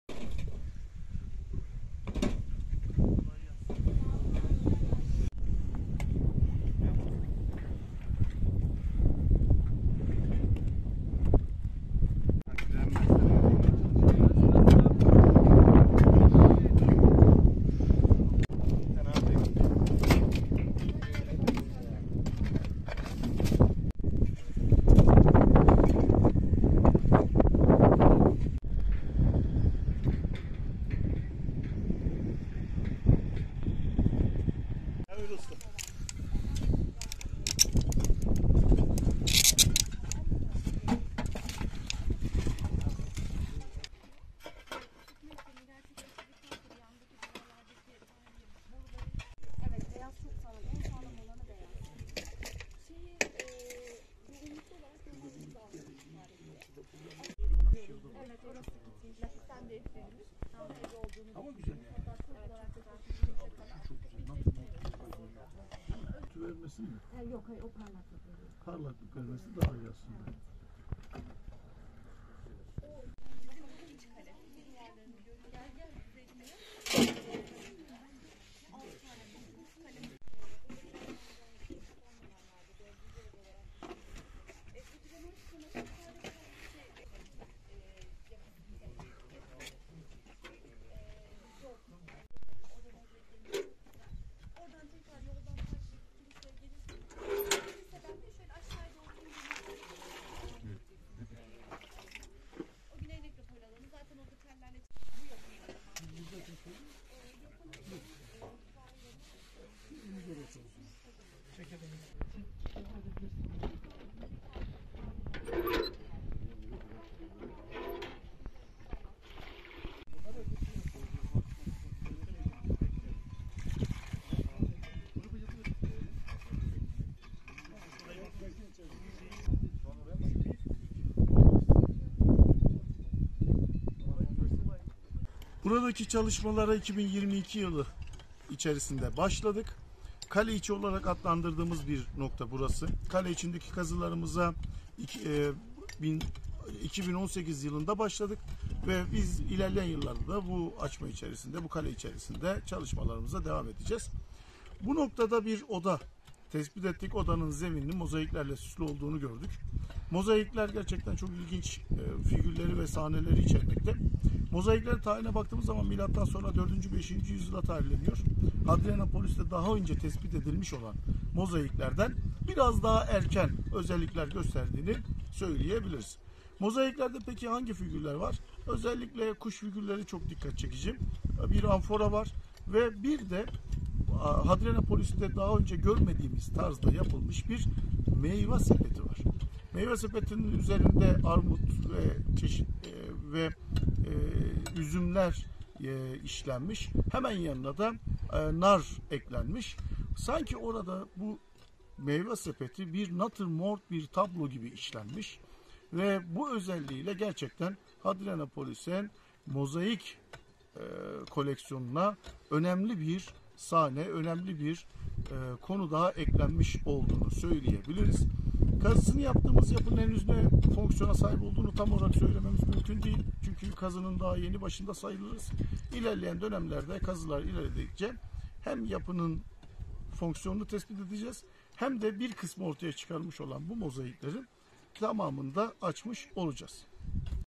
Hadrianopolis Kazılarında Yeni Mozaikler Bulundu
Karabük'ün Eskipazar ilçesindeki Hadrianopolis Antik Kenti'nde süren kazı çalışmalarında M.S. 4 ve 5'nci yüzyıla tarihlenen mozaikler bulundu. Kazı Başkanı Doç. Dr. Ersin Çelikbaş, "Meyve sepeti sanki natürmort bir tablo gibi işlenmiş. Bu özelliği ile Hadrianopolis'e, mozaik koleksiyonuna önemli bir sahne daha eklenmiş olduğunu söyleyebiliriz" dedi.
Yasin ALDEMİR/ESKİPAZAR (Karabük),(DHA)-